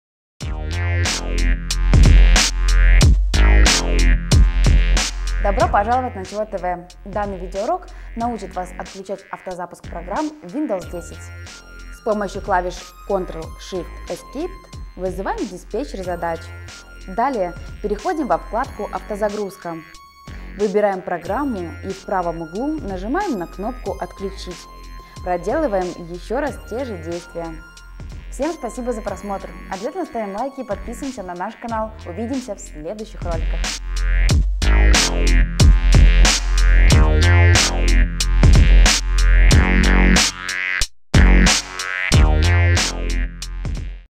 Как Отключить Автозапуск Программ в Windows 10 | Как Выключить Автозагрузку Программ в Windows 10
Данный видео урок научит Вас отключать автозапуск программ в Windows 10. С помощью клавиш Ctrl+Shift+Esc вызываем диспетчер задач. Далее переходим во вкладку “автозагрузка”. Выбираем программу и в правом углу нажимаем на кнопку “отключить”. Проделаем еще раз те же действия.